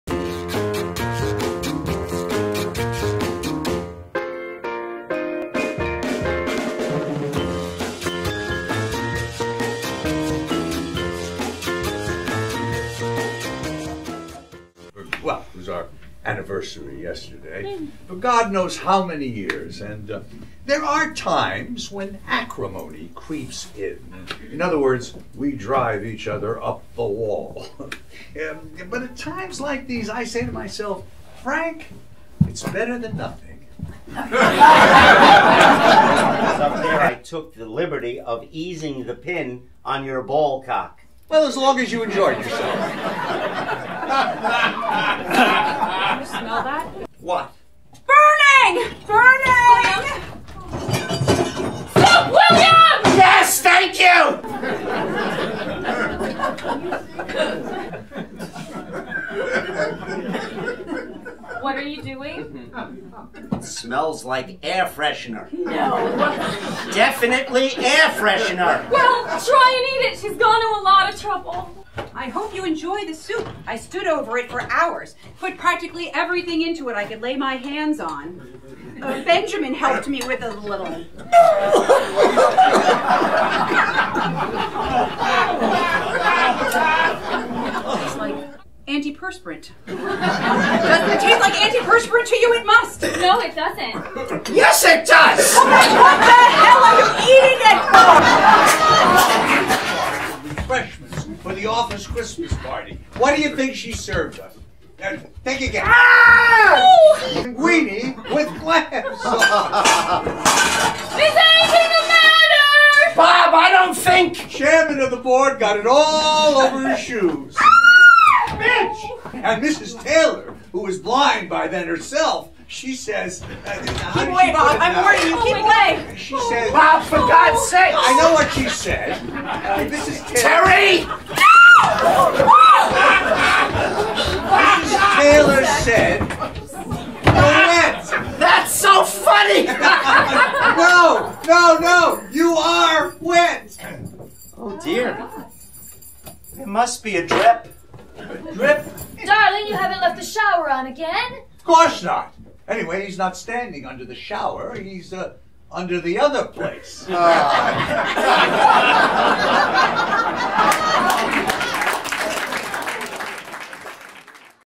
How The Other Half Loves excerpts from Act I
How the Other Half Loves by Alan Ayckbourn
Directed by Andrea Andresakis
At the Amateur Comedy Club, NYC, the longest continuously running theatre in the US
With:
Victoria Guthrie as Fiona
Charlie Moss as Frank
Sylvie Preston as Mary
Stan Goldberg as William
Liz Sanders as Teresa
Greg Gerard as Bob